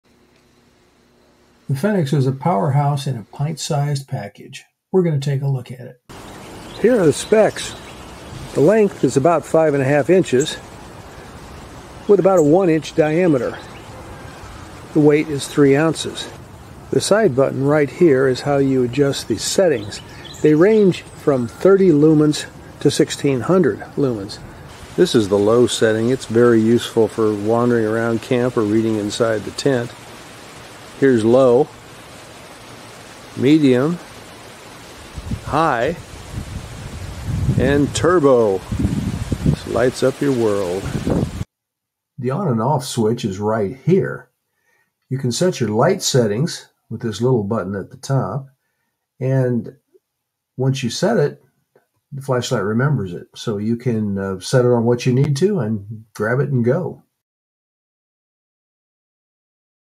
Check out the Fenix PD36R | A compact light with a big beam
Looking for a sturdy, reliable flashlight with a variety of light intensity settings? The Fenix PD36R might be just what you need. @survivalcommonsense

And we would really appreciate it if you would check out my book: "Bushcraft Basics: A Common Sense Wilderness Survival Handbook."